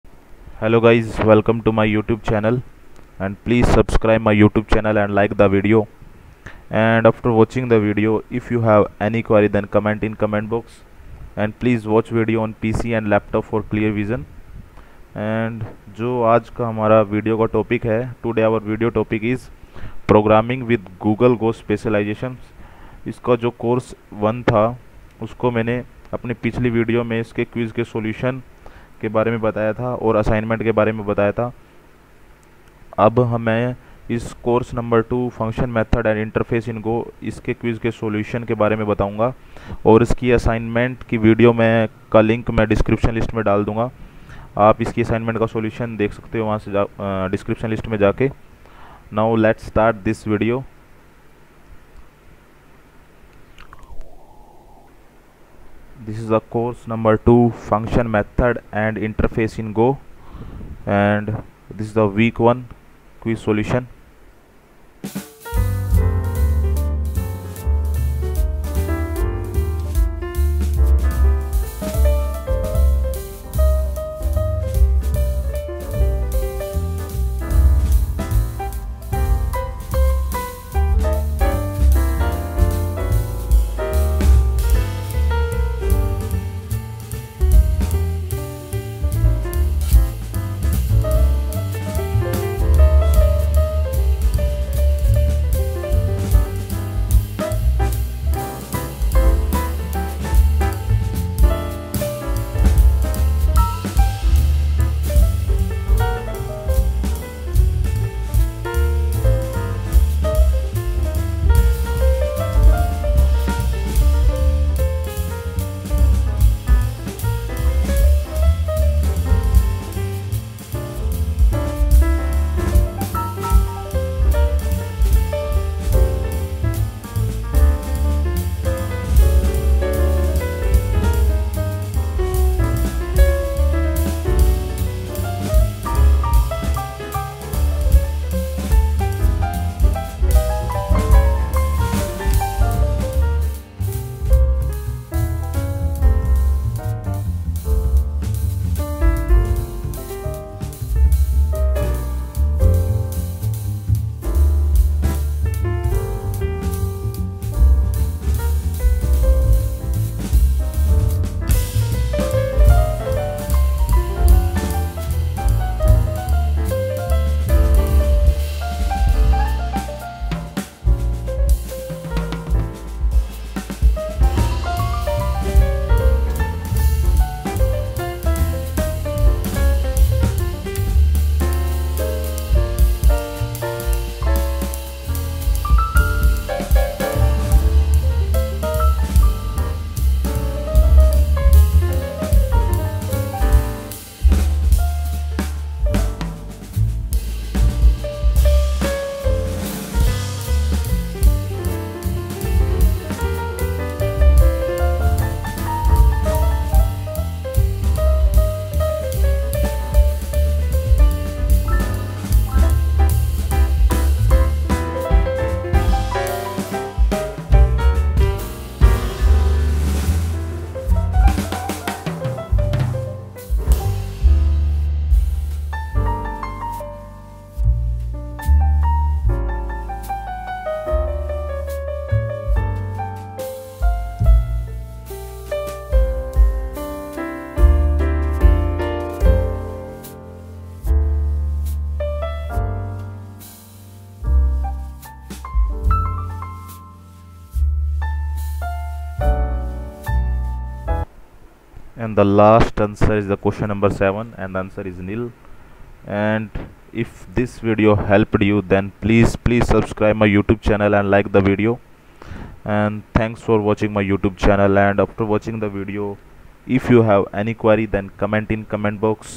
Functions, Methods, and Interfaces in Go all week quiz answer || Programming with google go answer
Continue your exploration of the Go programming language as you learn about functions, methods, and interfaces. Topics include the implementation of functions, function types, object-orientation in Go, methods, and class instantiation. As with the first course in this series, you’ll have an opportunity to create your own Go applications so you can practice what you’re learning.